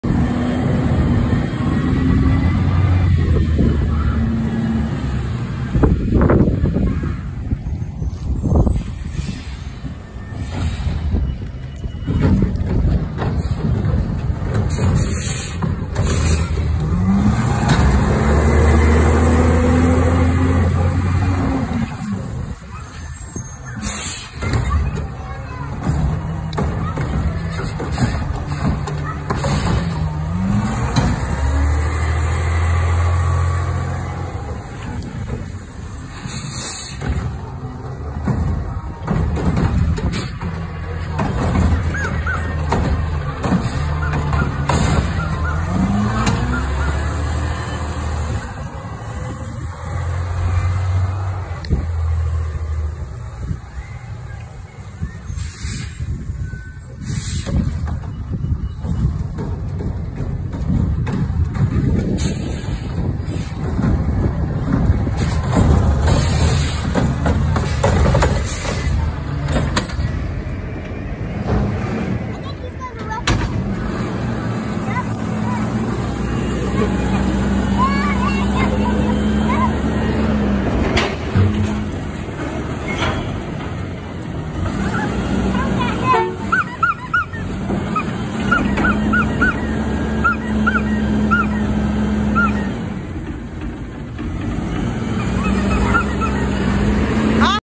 Advanced disposal heil Curotto can recycling truck ￼￼￼
Advanced disposal, recycling truck front loader, half back heil in action,  ￼￼￼